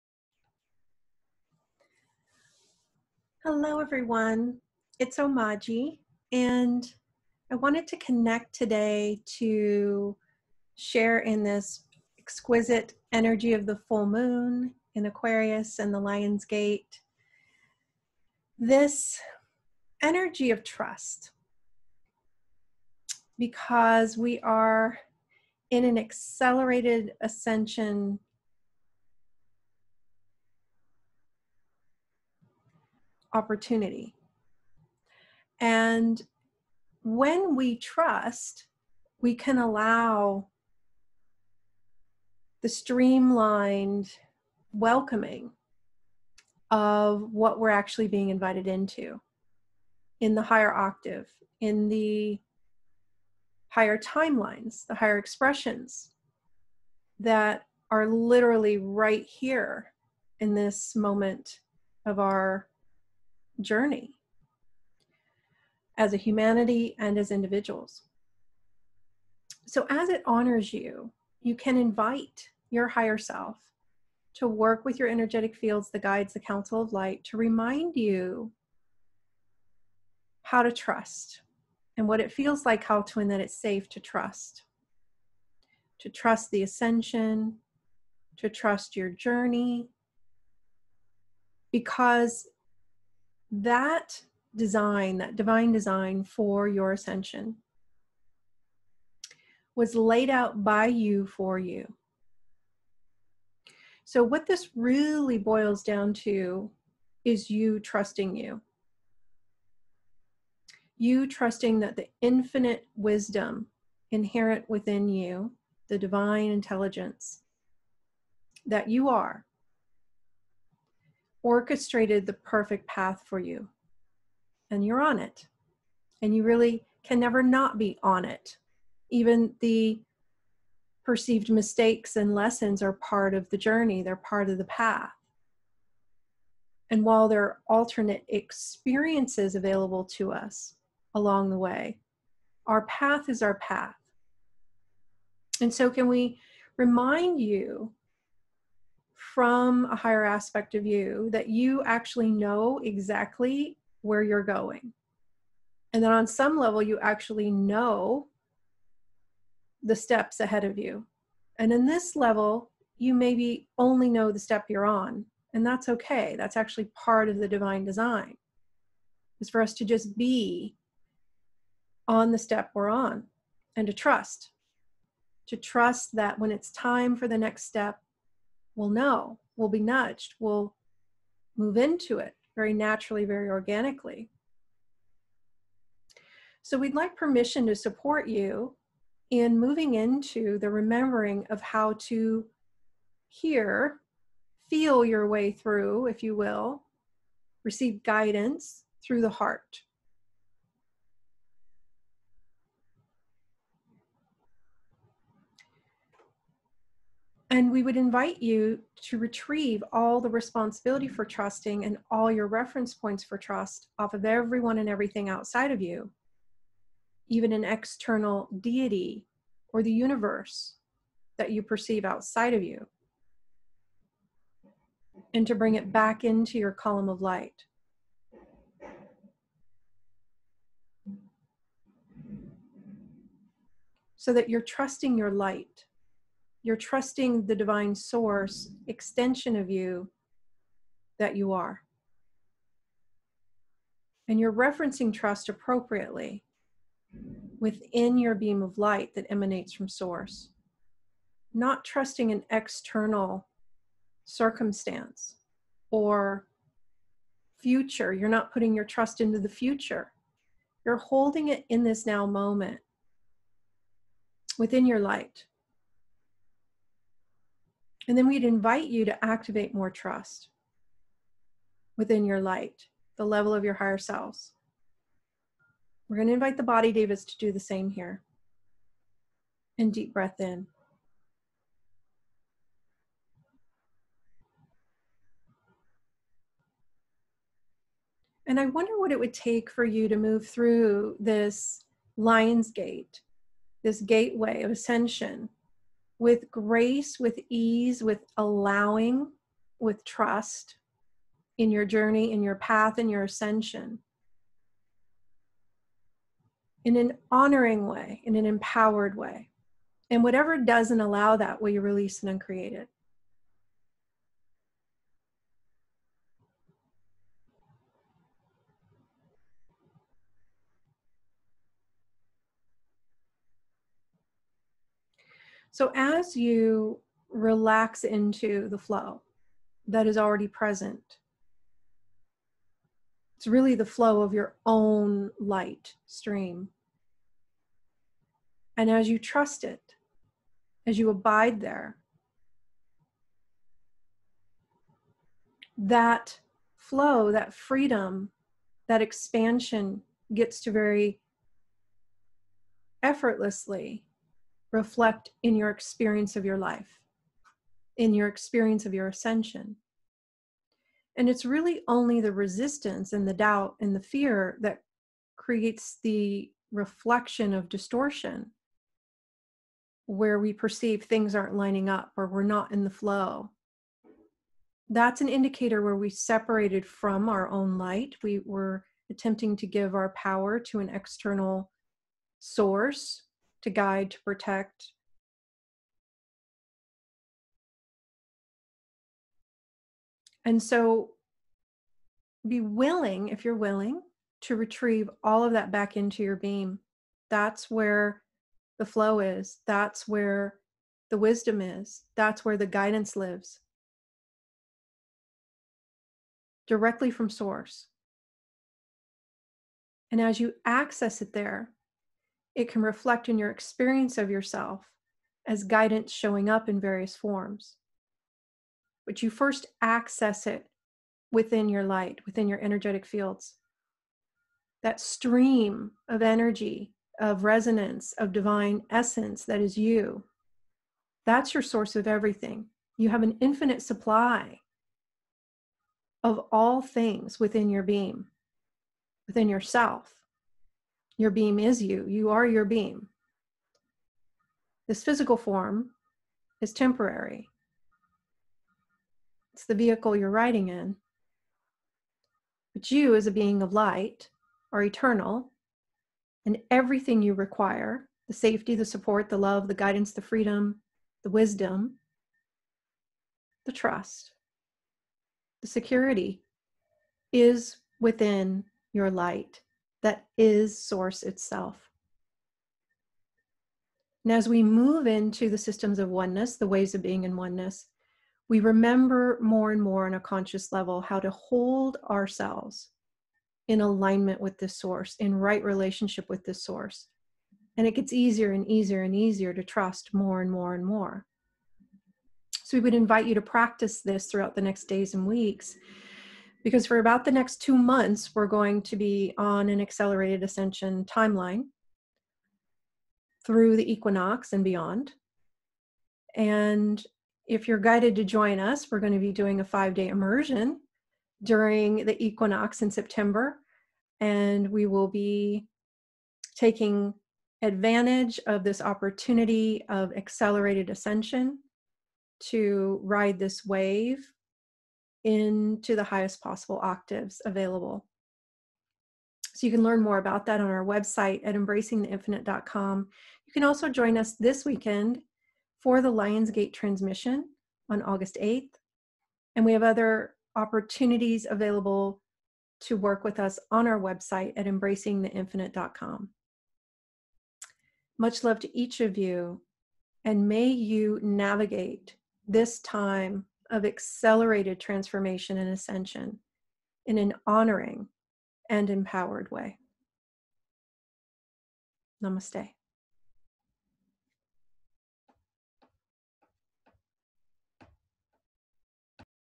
Activating Greater Trust
Will you Remember How to Trust from ONENESS and retire old separation based 'trust' mechanisms and configurations? Omaji and The Council of Light Support you in Remembering How to Trust your Light in Right Relationship with Source.